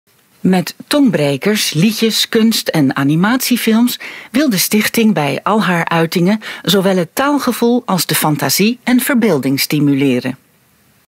Recensie Droomwevers Rappen met werkwoorden - NBD Biblion ingesproken door Yvonne Nasveld
“Met tongbrekers, liedjes, kunst en animatiefilms wil de stichting bij al haar
uitingen zowel het taalgevoel als de fantasie en verbeelding stimuleren.”
S. Mulders, NBD Biblion
ingesproken door Yvonne Nasveld
opgenomen bij Studio Bart de Groot in Amsterdam